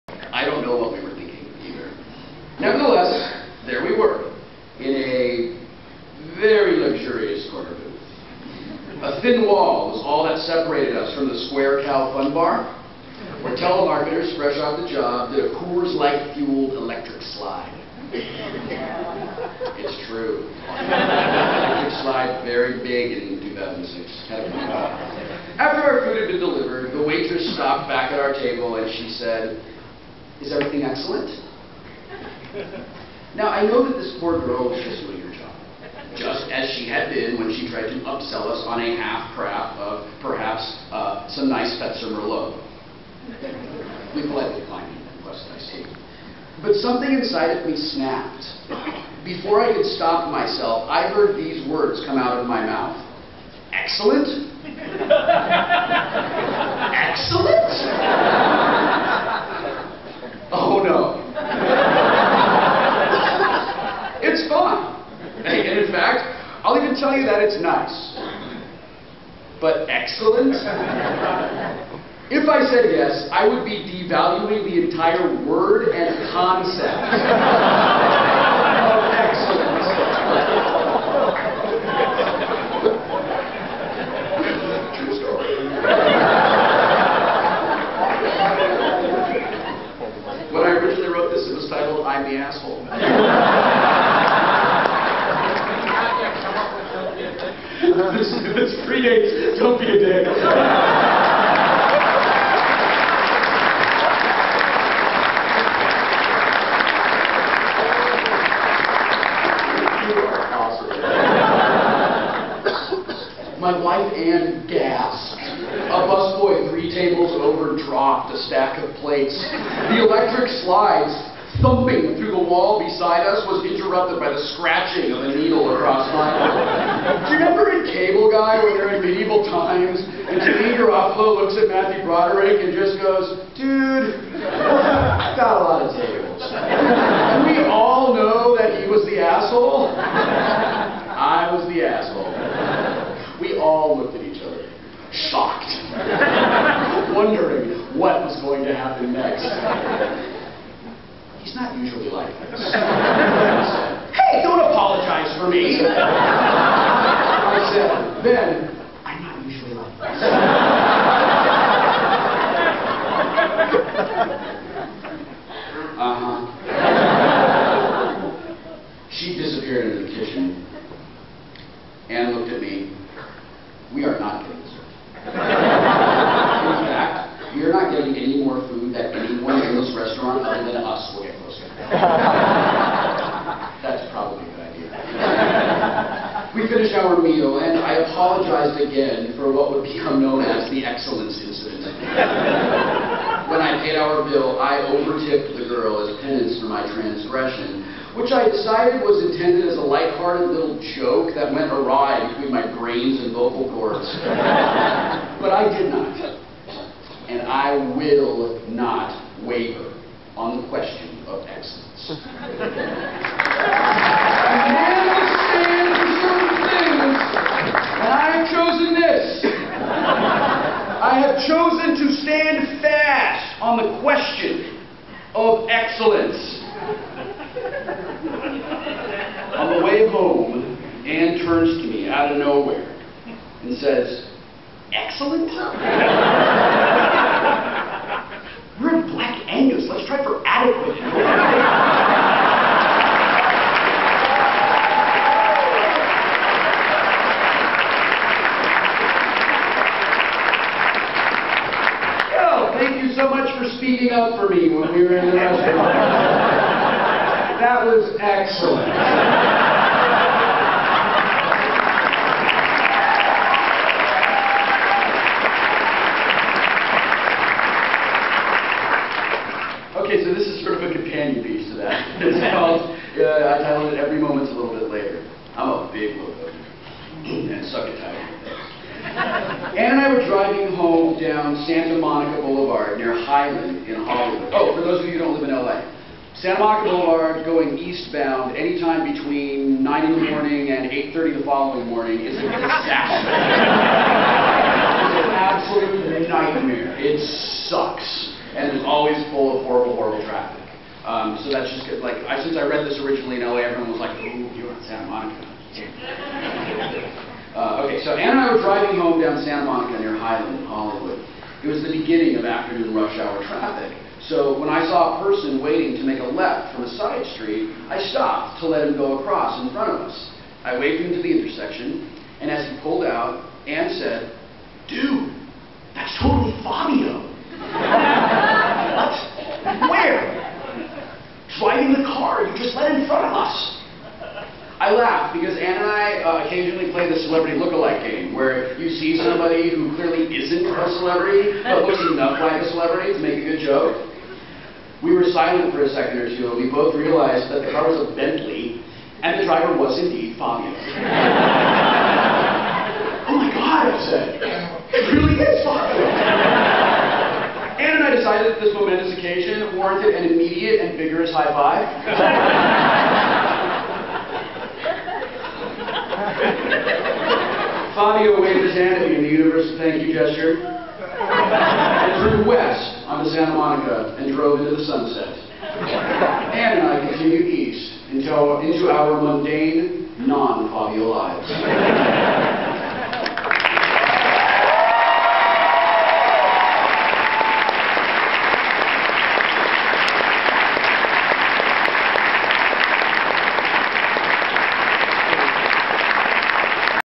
Wil Wheaton - PAX Prime 2009.
Wil Wheaton reads two pieces for your listening enjoyment:  "the excellence incident" and "every moment is a little bit later".

Filmed at "the Wil Wheaton presents: THE AWESOME HOUR!!" during the 2009 PAX Prime in Seattle, Washington.